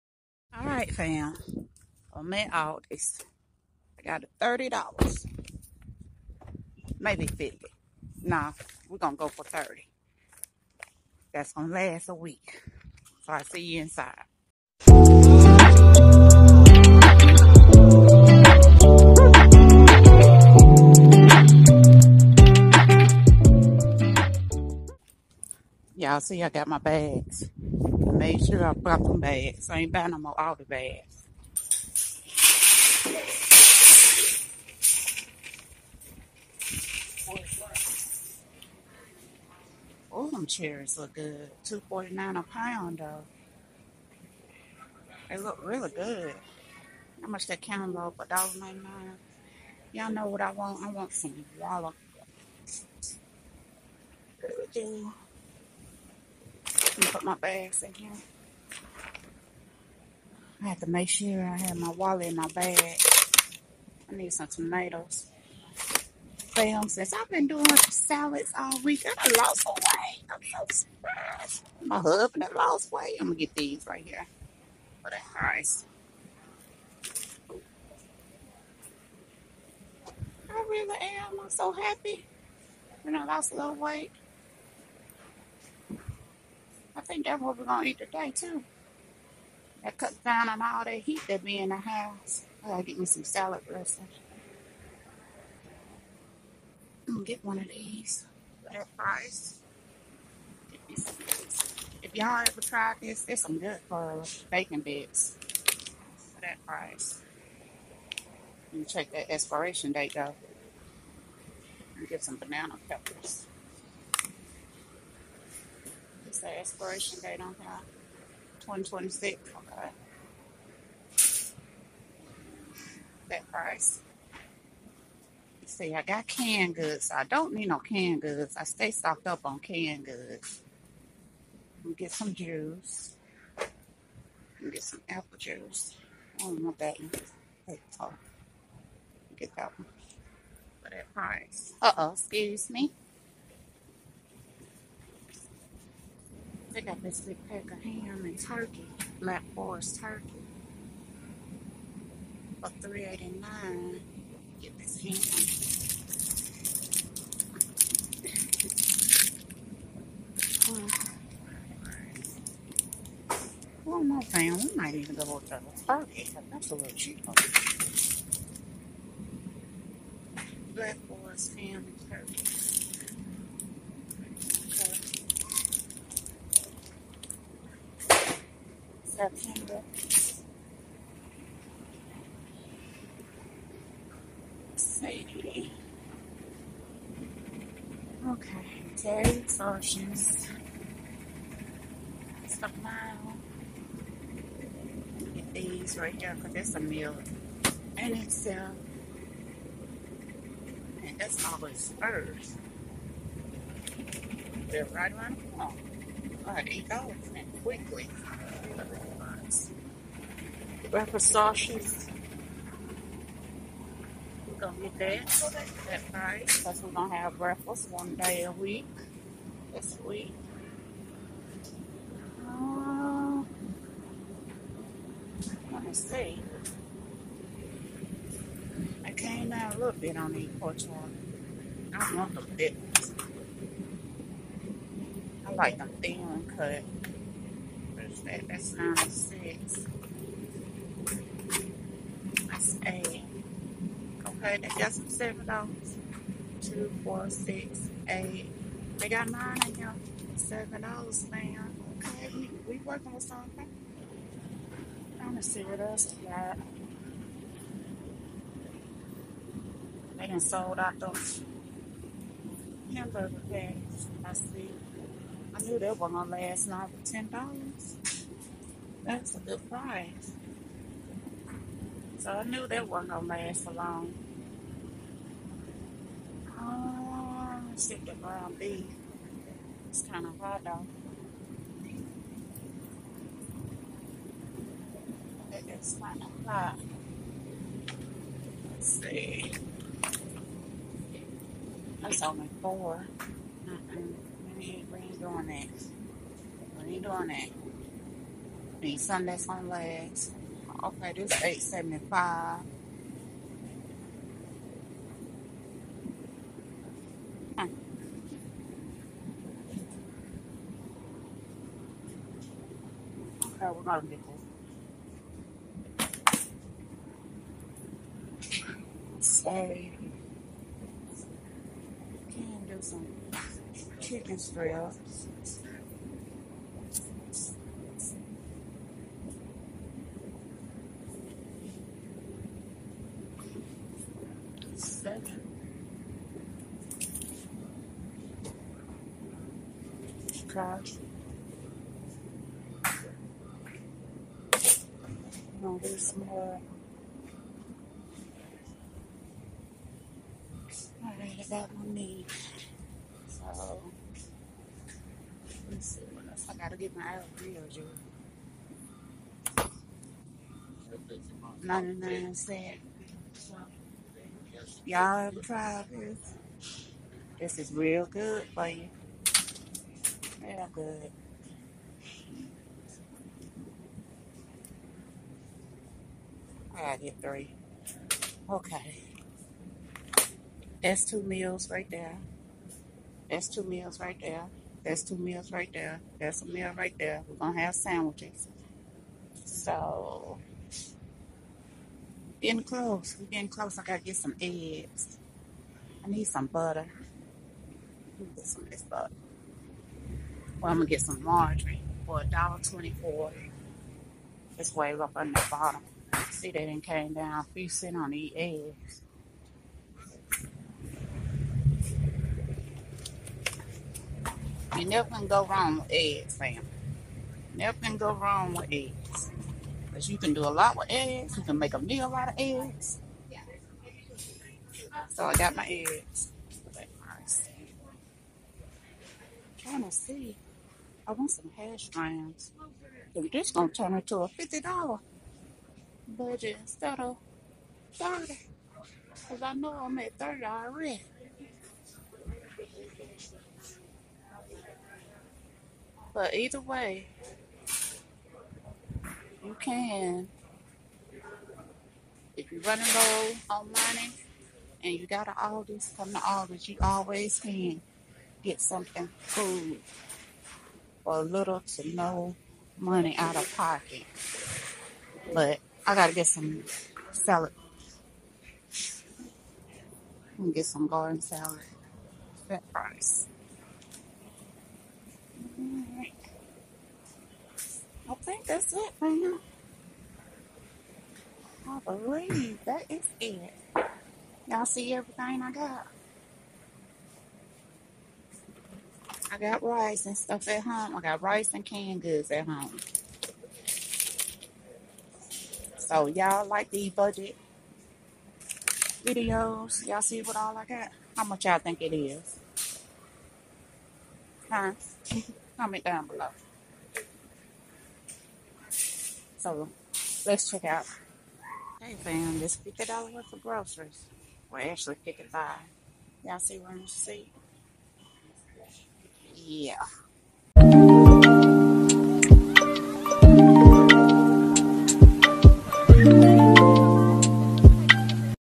I Spent $50 At Aldi • Grocery Shopping Challenge
Join me as I take on the $50 grocery shopping challenge at Aldi! See what I can buy on a budget and how I make the most of my money at this budget-friendly grocery store.